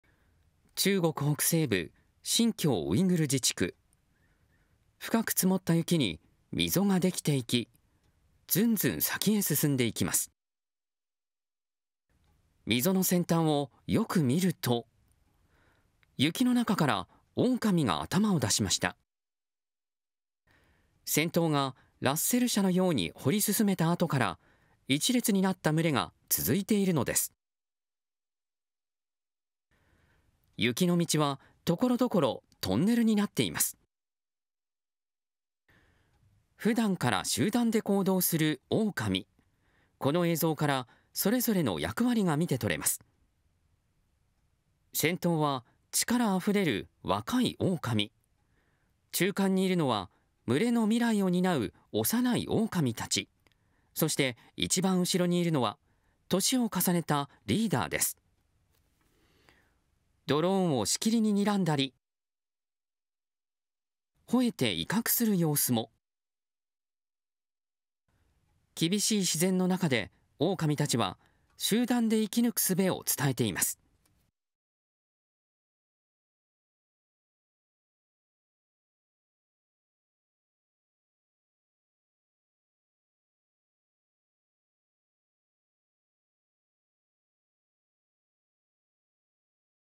雪の中 オオカミが移動 群れでの役割 溝に反映 中国
中国北西部、新疆ウイグル自治区。
深く積もった雪に溝ができていき、ずんずん先へ進んでいく。

溝の先端をよく見ると、雪の中からオオカミが頭を出した。

先頭がラッセル車のように掘り進めたあとから、1列になった群れが続いている。

雪の道は、ところどころトンネルになっている。

普段から集団で行動するオオカミ。
この映像から、それぞれの役割が見て取れる。

先頭は、力あふれる若いオオカミ。
中間にいるのは、群れの未来を担う幼いオオカミたち。
そして、一番後ろにいるのは、年を重ねたリーダー。

ドローンを、しきりににらんだり、ほえて威嚇する様子も。

厳しい自然の中で、オオカミたちは集団で生き抜くすべを伝えている。